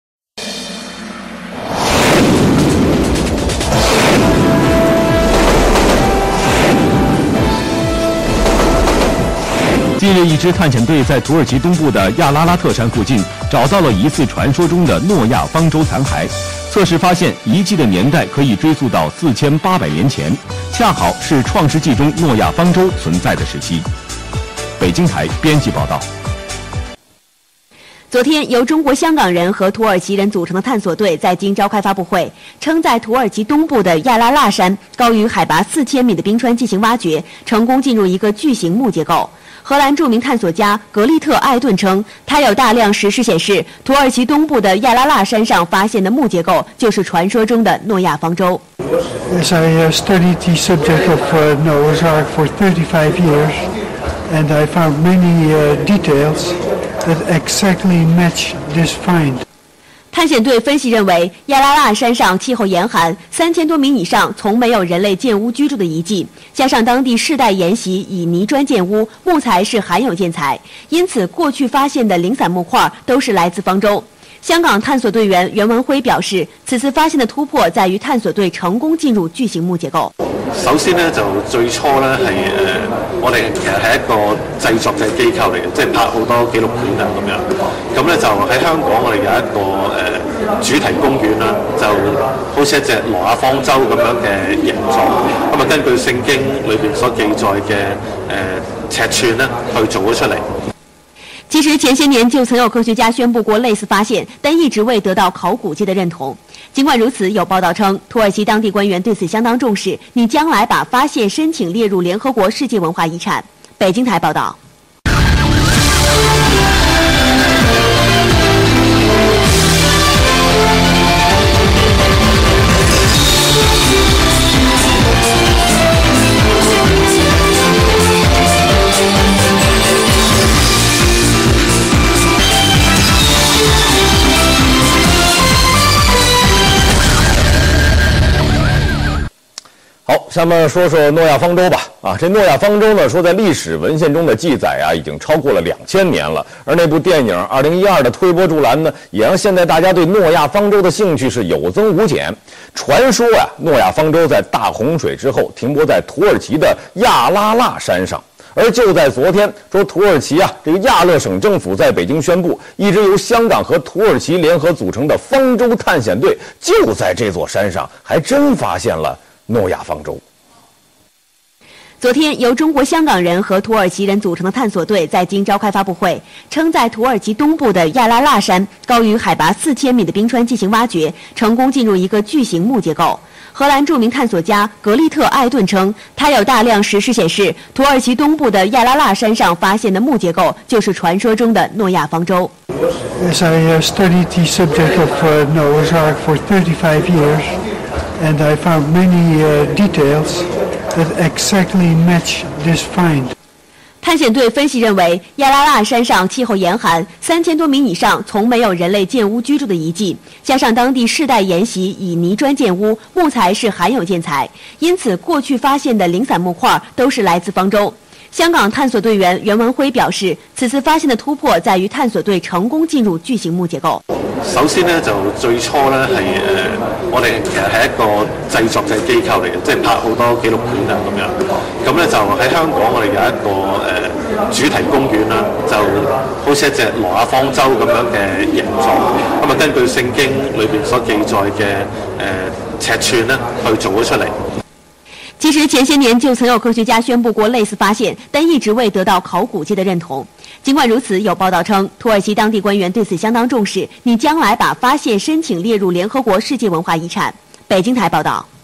China News reports on discovery Noah's ark
China News Channels, BTV and CCTV, reports on discovery Noah's ark. 28/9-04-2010